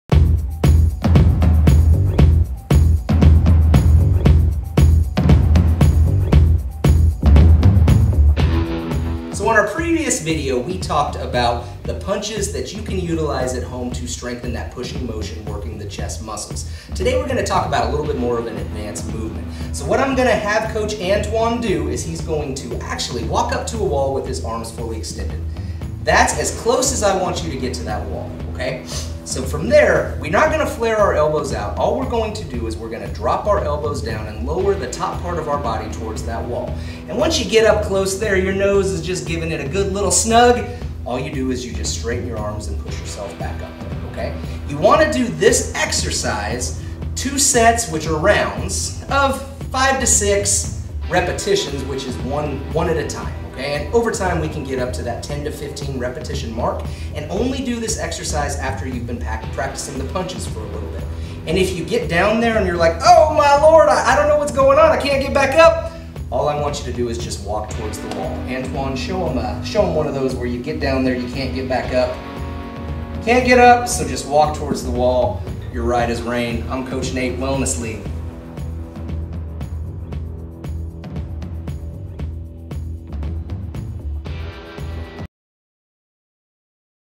Move of the Week: PUSH Advanced Movement
In follow-up to yesterday's more basic movements, today we're showing you a slightly more advanced movement that you can still do right from the comfort of your own home to build strength and be the best you can be!